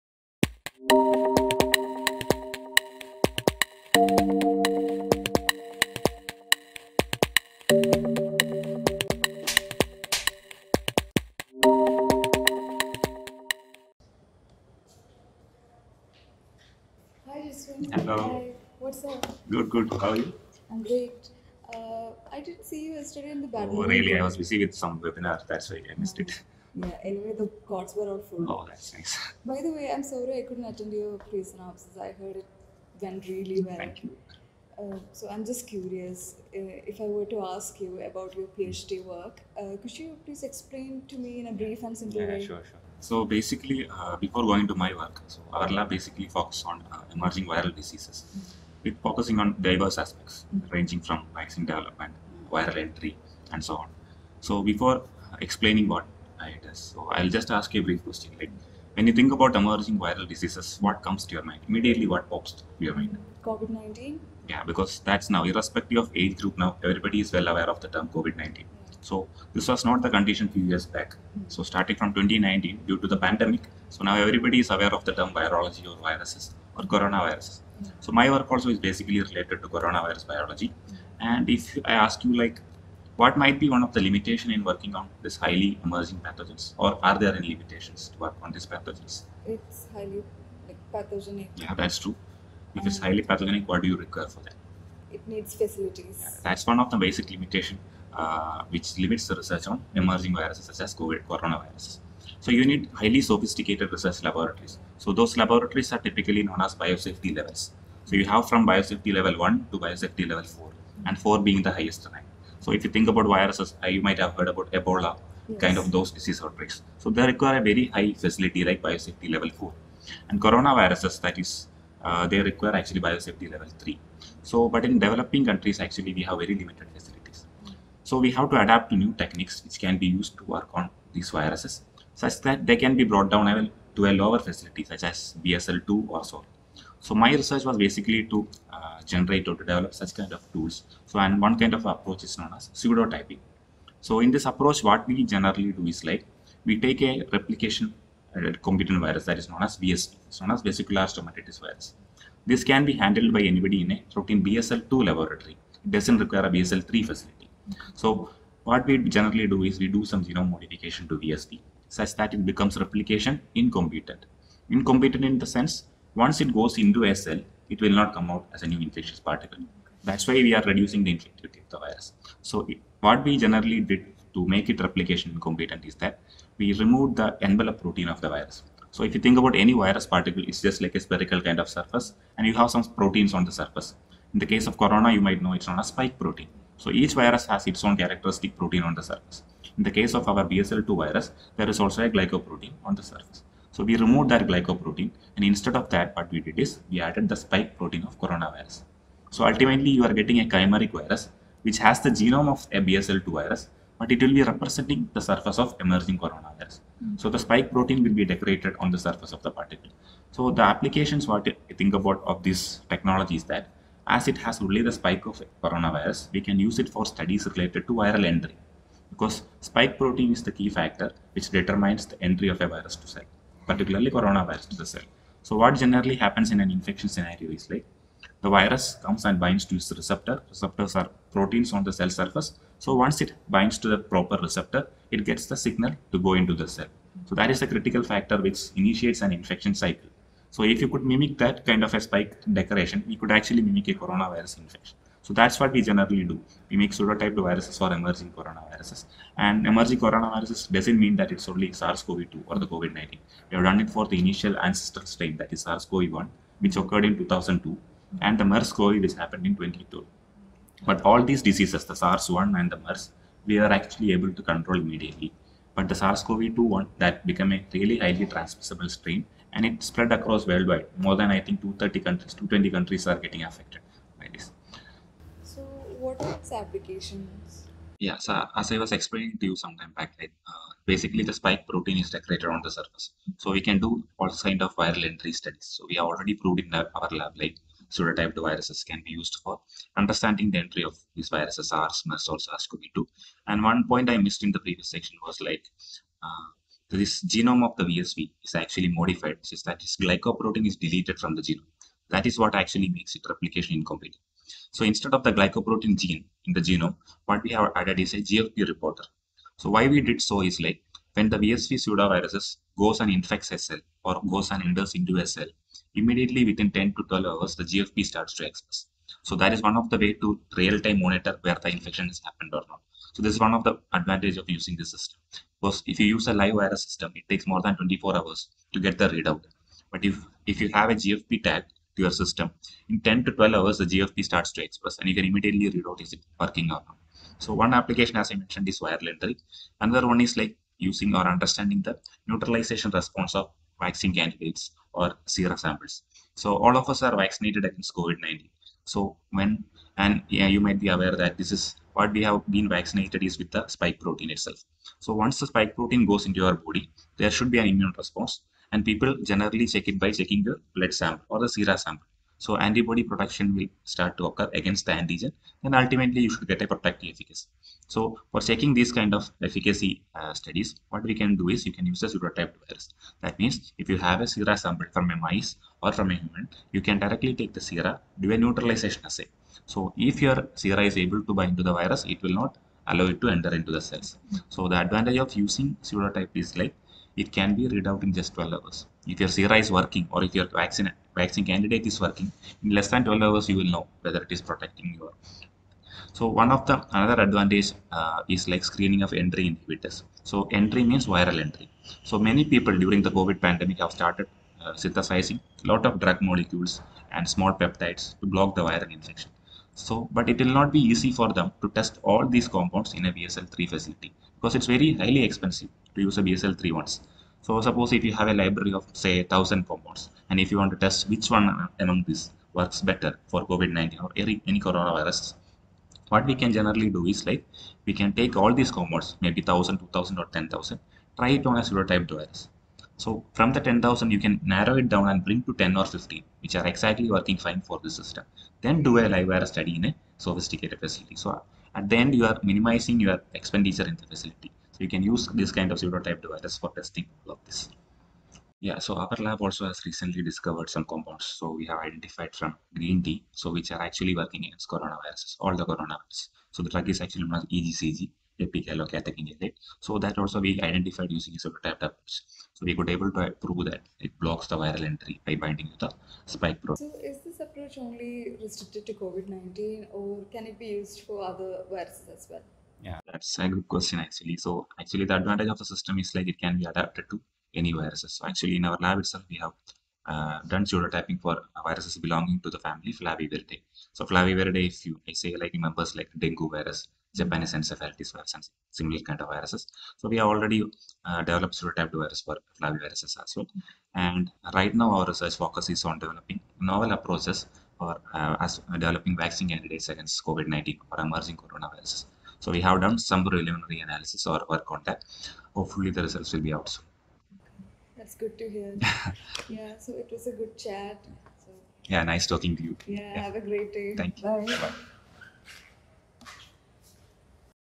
PITCH PLEASE! | Ep. 3 | In conversation with Dr Jeswin Joseph @IntoTheirMindsITM
How well-informed is the general public about a scientist's research? Gaining insights from years of work can be daunting to the average person, and information about all the amazing research going on around us seems hidden from the public eye.

So, what if we approached a researcher and asked them to pitch their work in a short but accessible manner?

For that very reason, we at @IntoTheirMindsITM  decided to come up with a new series, 'Pitch Please!'. Here, you get to watch an elevator pitch (minus the elevator) with an interesting researcher and get a glimpse of their work.

In our third episode, we have Dr Jeswin Joseph, who completed his PhD recently from the School of Biology, IISER Thiruvananthapuram. He offers us a peek into his world of Virology and finding therapeutics against the infamous Covid-19.

Featuring: Dr Jeswin Joseph and Sreelakshmi C A
Video recording: Gopal Kulkarni
Production: Palak Chawla, Aditya Naresh and Sreelakshmi C A